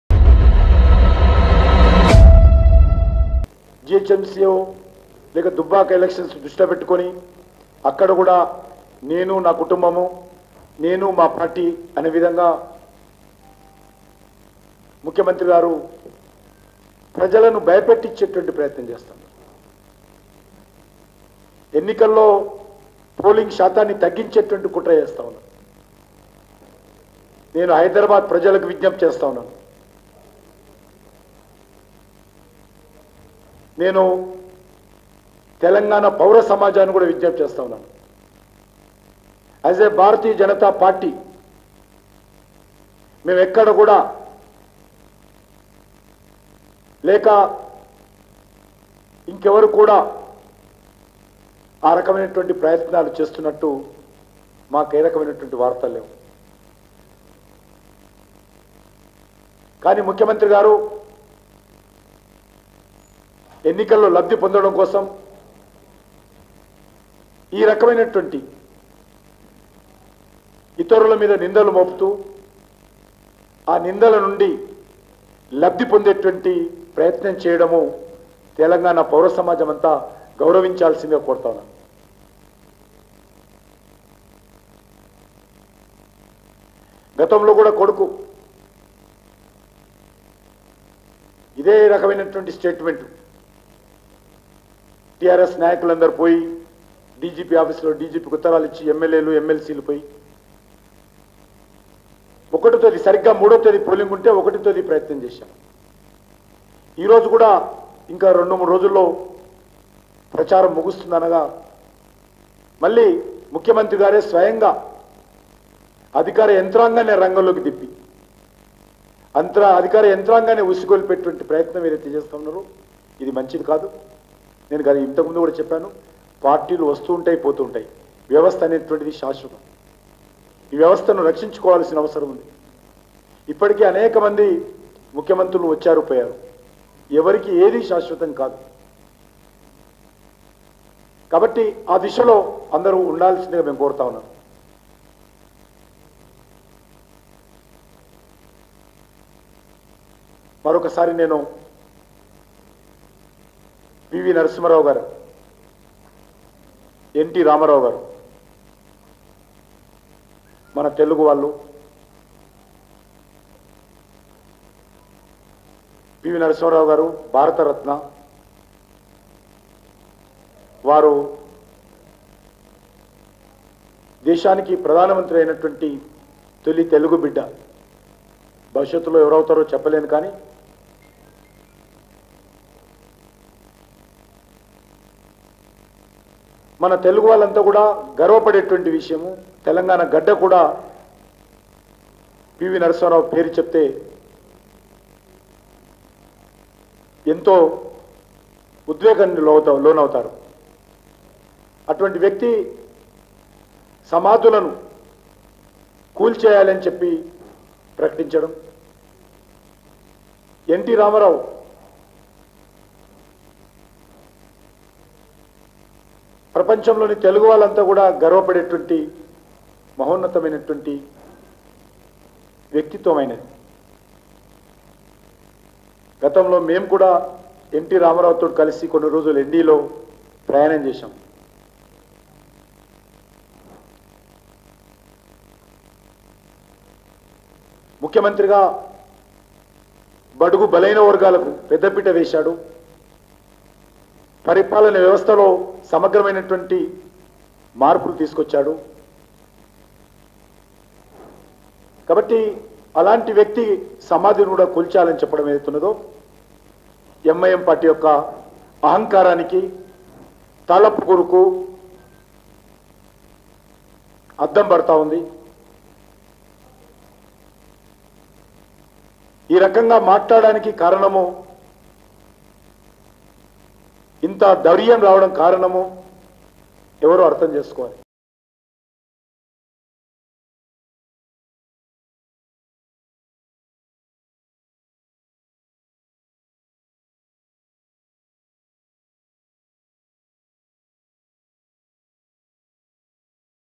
BJP MP Kishan Reddy Comments On KCR Over Dubbaka Elections 2020 | GHMC Elections 2020 | Hybiz Telugu
Watch BJP MP Kishan Reddy Comments On KCR Over Dubbaka Elections 2020 | GHMC Elections 2020 | Hybiz Telugu